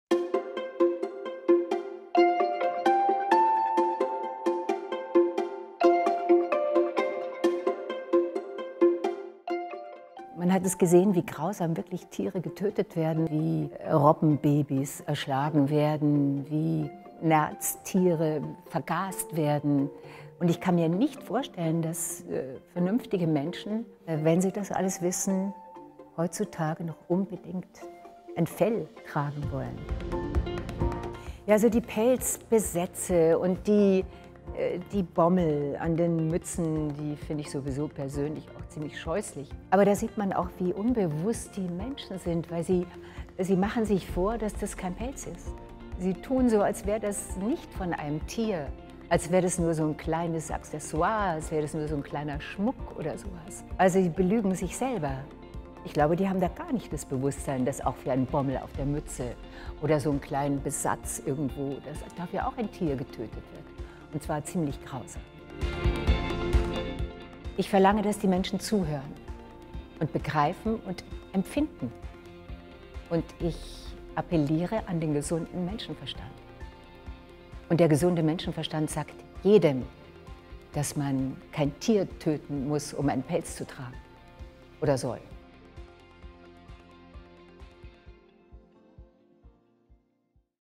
Hannelore Elsner: So trägt man Pelz / PETA
Schmusen zum Wohl der Tiere: Schauspielerin Hannelore Elsner und Hundedame Lisa schmiegen sich innig aneinander. „So trägt man Pelz!“ lautet die Botschaft der PETA-Kampagne. Gemeinsam mit der Tierrechtsorganisation zeigt die gefeierte Filmdiva, dass Pelz den Tieren gehört. Im Making-of-Video erklärt sie, warum Mode und Tierleid niemals Hand in Hand gehen dürfen.